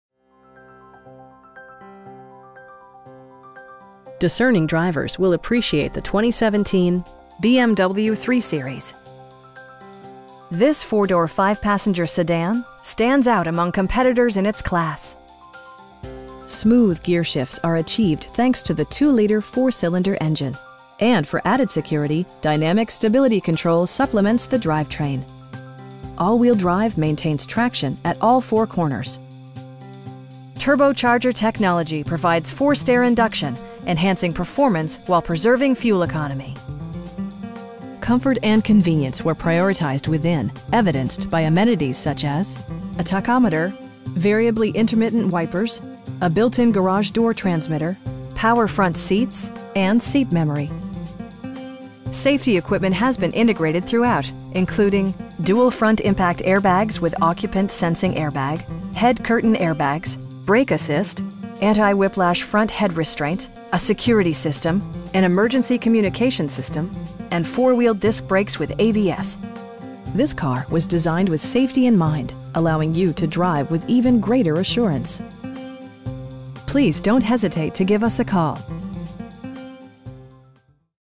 2017 BMW 330i xDrive in Portland, OR 97201
BMW Portland
2001 SW Jefferson St

Discerning drivers will appreciate the 2017 BMW 3-Series. This 4 door, 5 passenger sedan stands out among competitors in its class! Smooth gearshifts are achieved thanks to the 2 liter 4 cylinder engine, and for added security, dynamic Stability Control supplements the drivetrain. All wheel drive maintains traction at all four corners. Turbocharger technology provides forced air induction, enhancing performance while preserving fuel economy. Comfort and convenience were prioritized within, evidenced by amenities such as: a tachometer, variably intermittent wipers, a built-in garage door transmitter, power front seats, telescoping steering wheel, power windows, and seat memory. Safety equipment has been integrated throughout, including: dual front impact airbags with occupant sensing airbag, head curtain airbags, brake assist, anti-whiplash front head restraints, a security system, an emergency communication system, and 4 wheel disc brakes with ABS. This car was designed with safety in mind, allowing you to drive with even greater assurance. You will have a pleasant shopping experience that is fun, informative, and never high pressured. Please don't hesitate to give us a call.